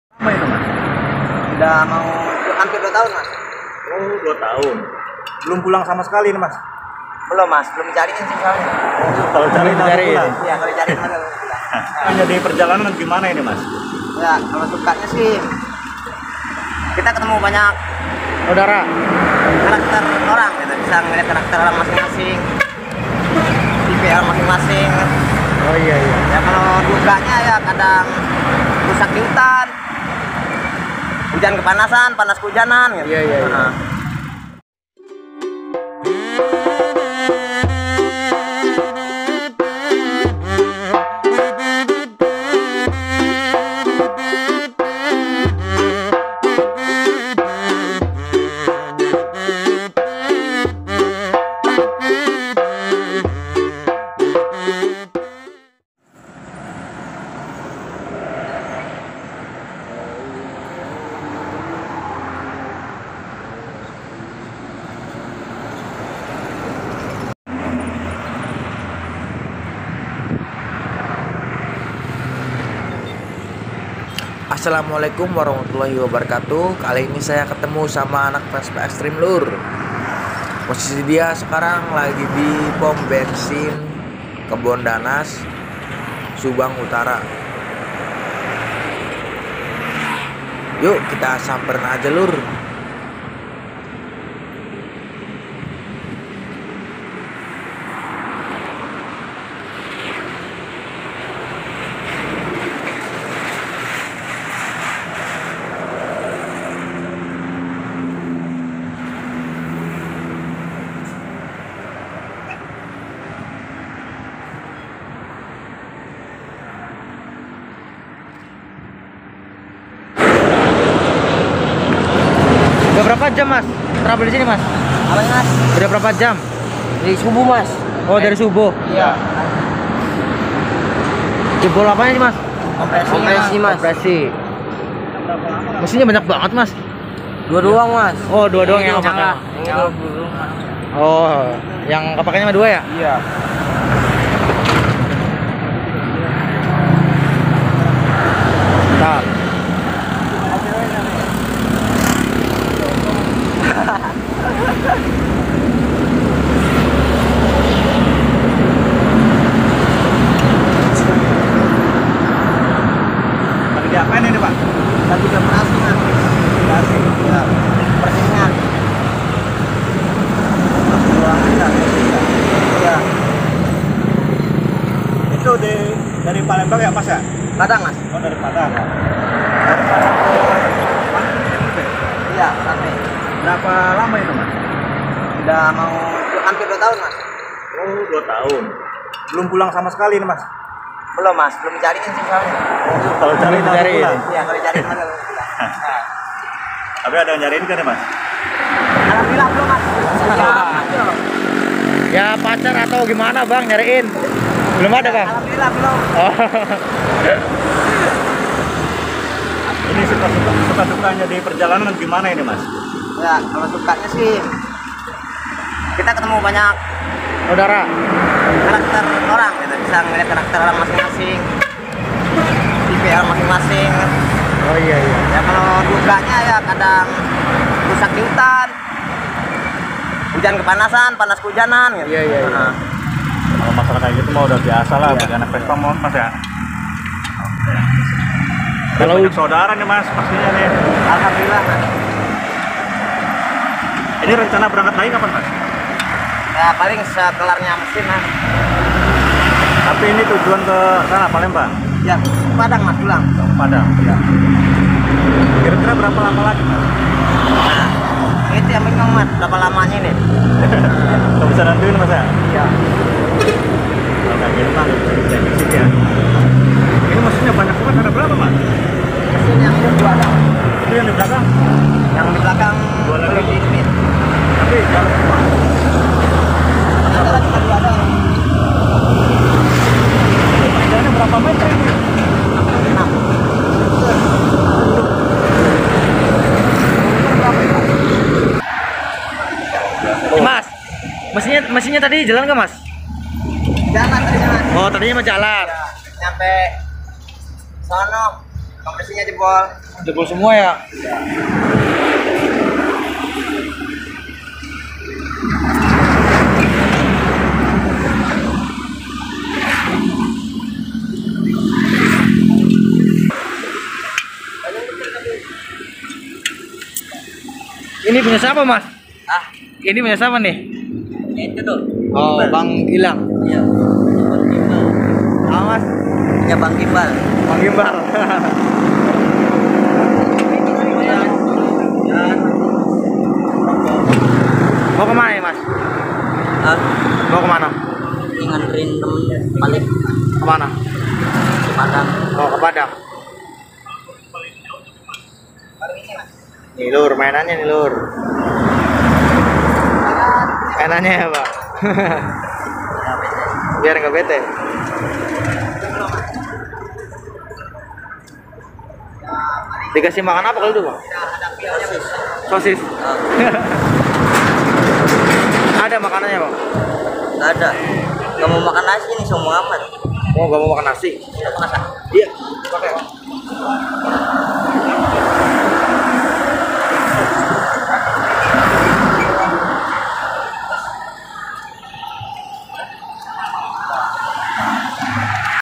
ANAK VESPA EXTREME 2 TAHUN BELUM PULANG SUKA DUKA PAHIT MANISNYA HIDUP DI JALANAN MEREKA UDAH BIASA
Selamat Menonton Semoga Kalian Sehat Selalu Dan Di Beri Umur Panjang Amin